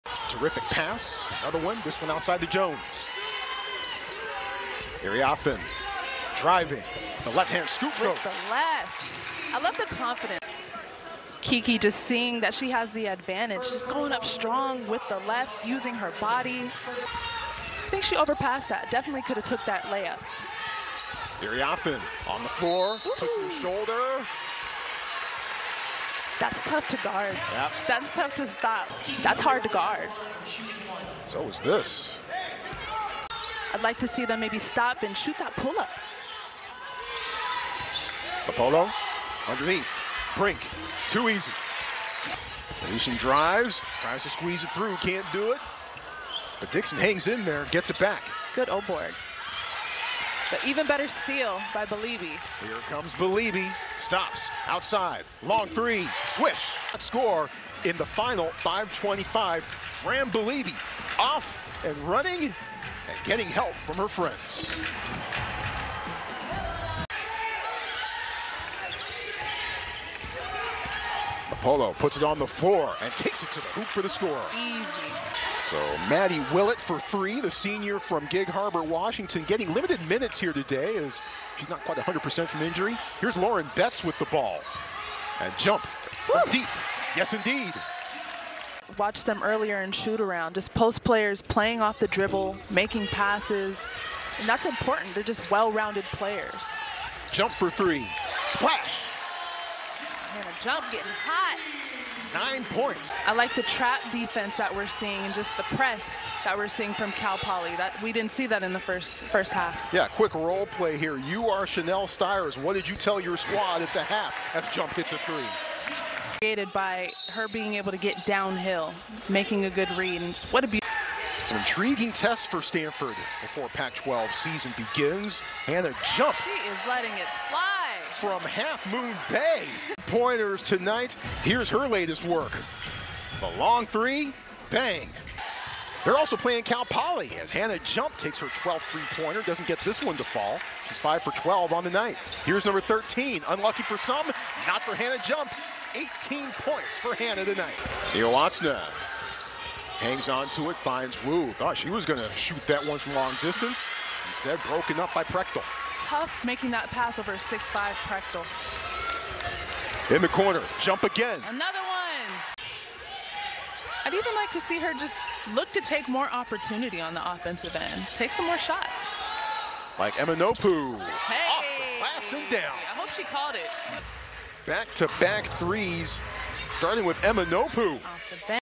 Stanford Women's Basketball vs. Cal Poly
Hannah Jump matched her career high with a season-best 24 points and also equaled her career best of eight 3-pointers, leading the Cardinal past Cal Poly 80-43 on Wednesday night.

Twitter.com/GoStanford
Instagram.com/GoStanford
Facebook.com/StanfordAthletics